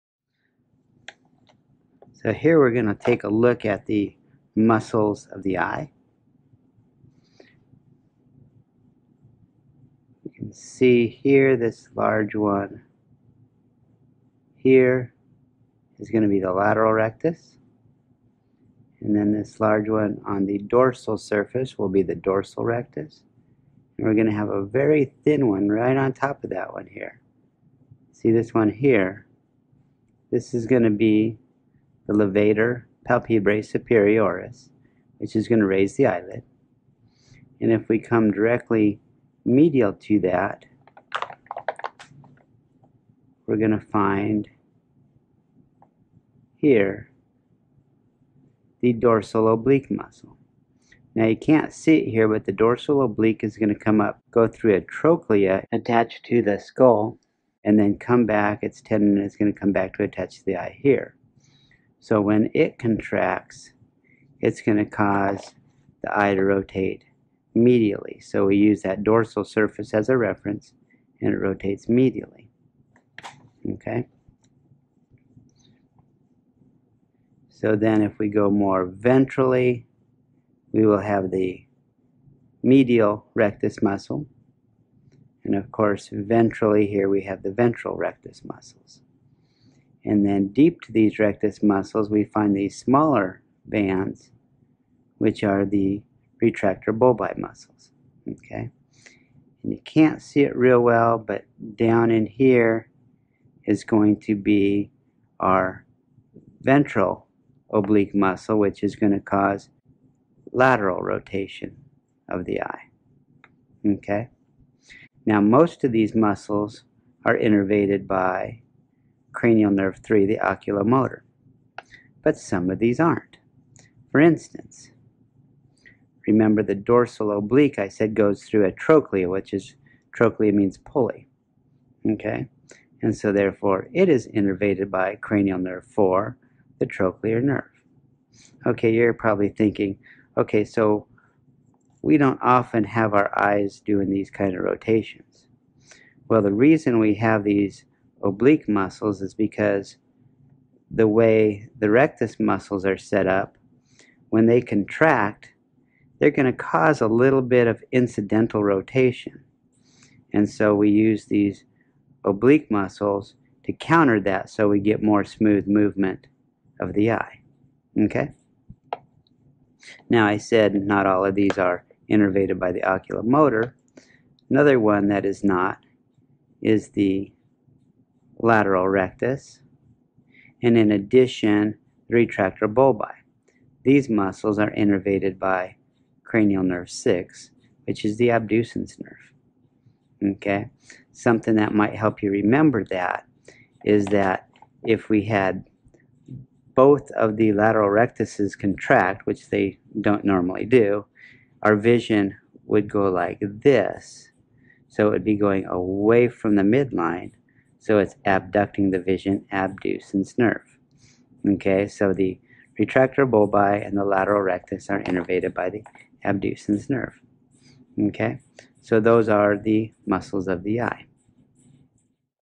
Muscles of Eye Movement
This video examines the extrinsic muscles of the eye and discusses their innervation. If you find this helpful, please let me know by "Like" it. Thanks!